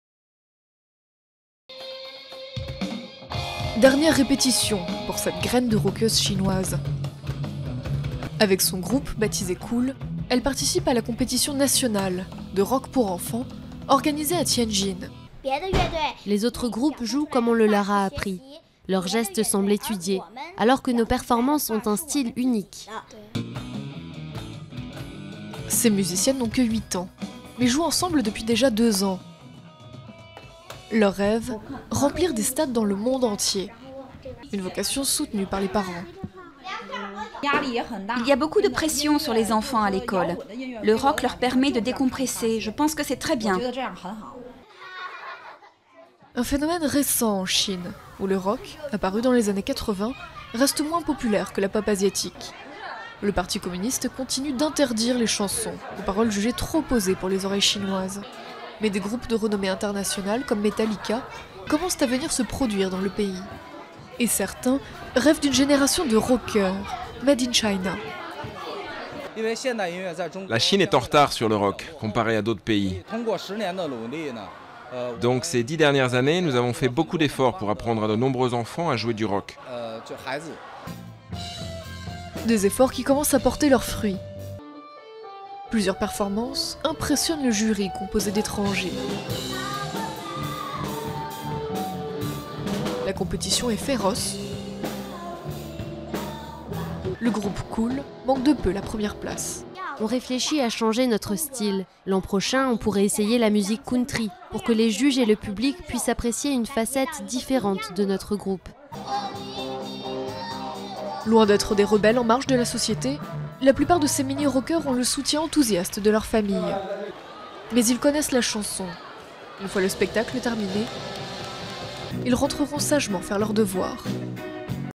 Chine: des écoliers et rockeurs en herbe rêvent de gloire
Des enfants chinois s'affrontent lors d'une compétition de concerts de rock annuelle. Durée: 02:24